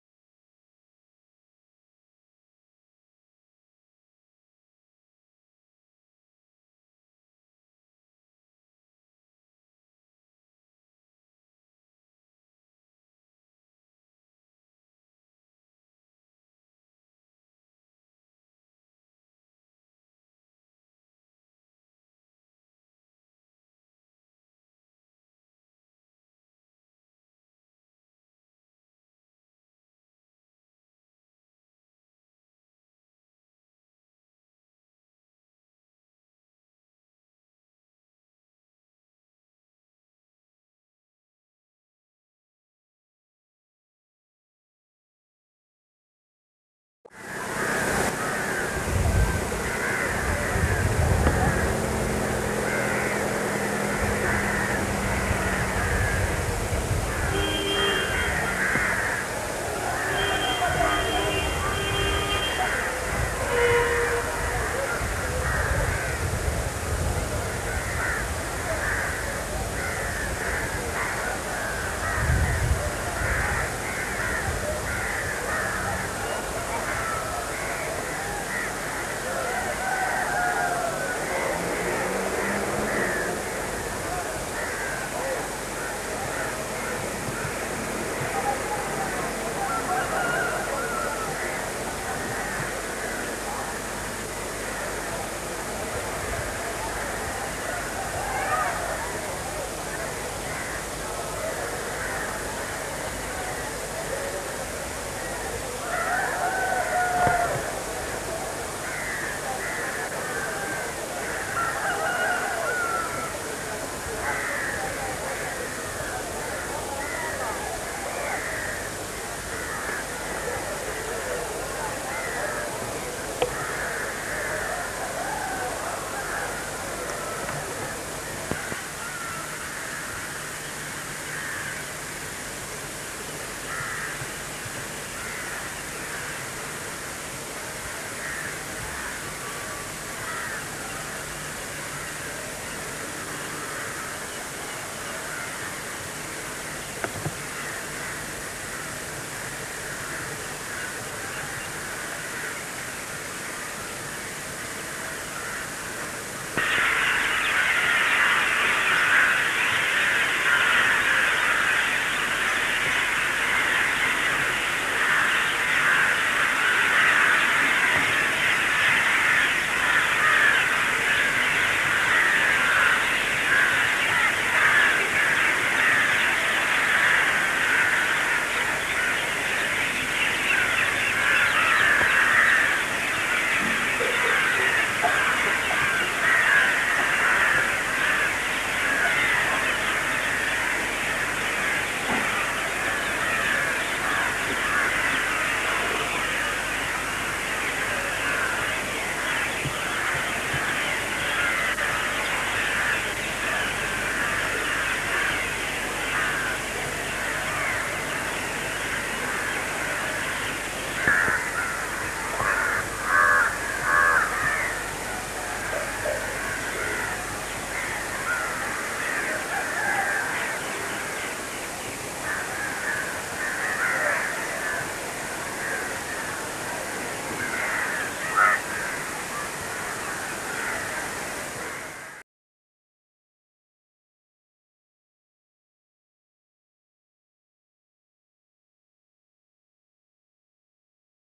Puttaparthi Amanecer Divino_1989.
Viaje a India, recorrido por el Ashram de Sai Baba Prasanthi Nilayam y sus alrededores en Puttaparthi. Relatado por Anne Rodriguez Barfoot -Argentina-, filmado por Renzo Zanutel y Martina de Col -Italia- y editado y difundido por SAI BABA TV y SATHYA SAI BABA YOU TUBE..